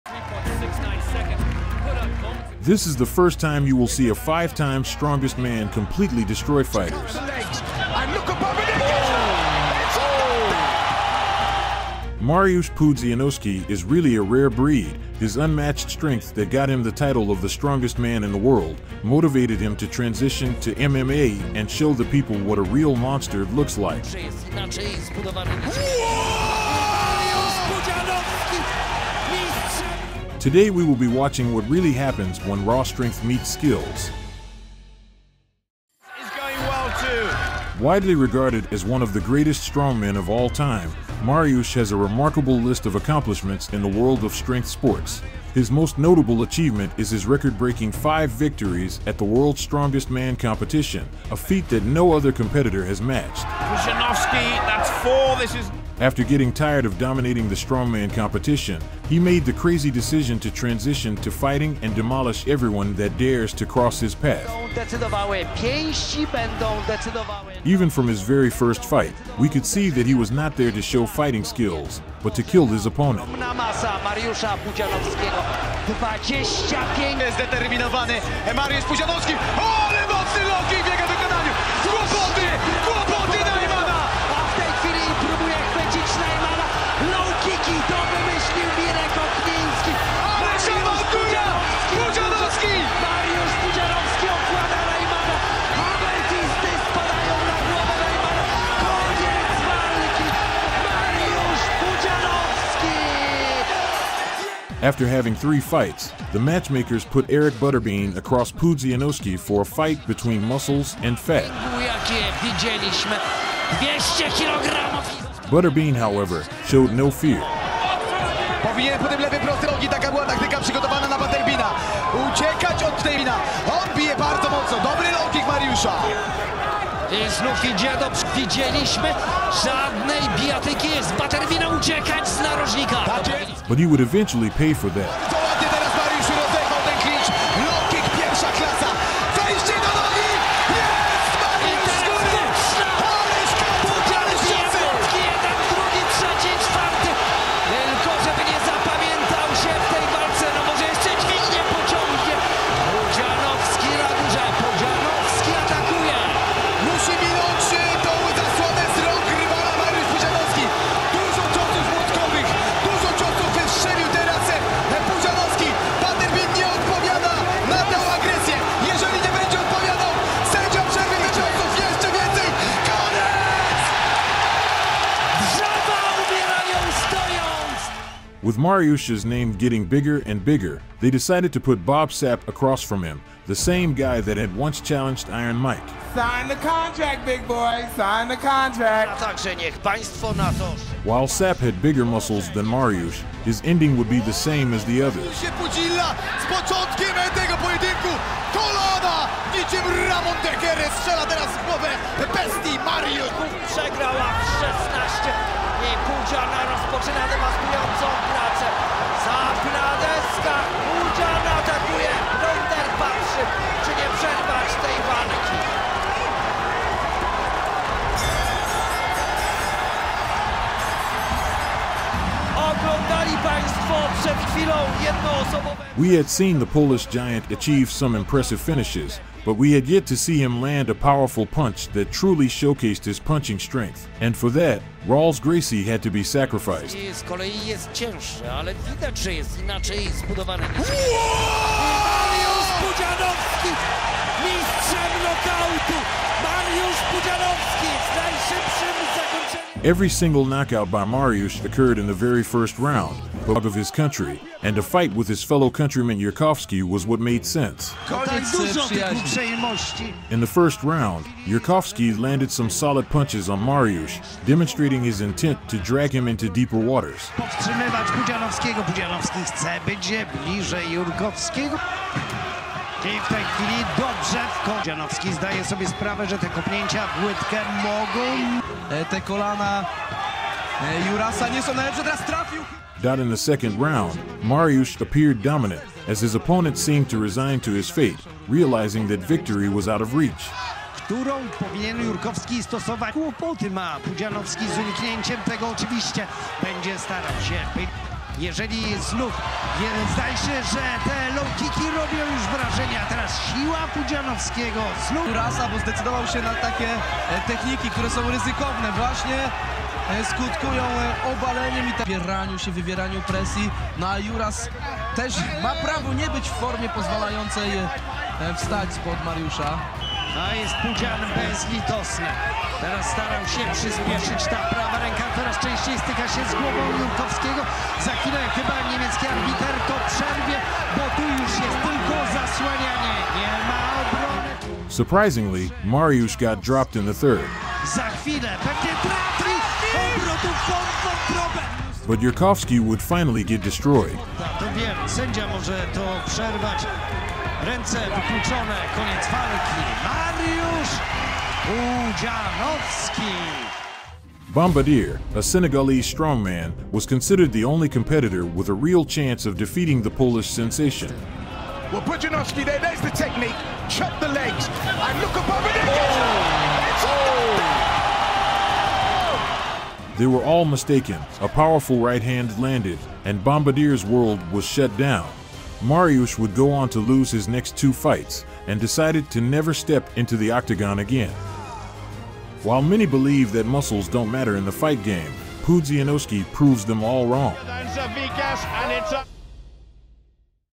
When 5x Strongest Man DESTROYED Cocky Fighters For Being Disrespectful! - Mariusz Pudzianowski
This is the first time you will see a 5 times strongest man completely destroy fighters.

Mariusz Pudzianowski is really a rare breed, his unmatched strength that got him the title of the strongest man in the world motivated him to transition to mma and show the people what a real monster looks like.

Today we will be watching what really happens when raw strength meets skills.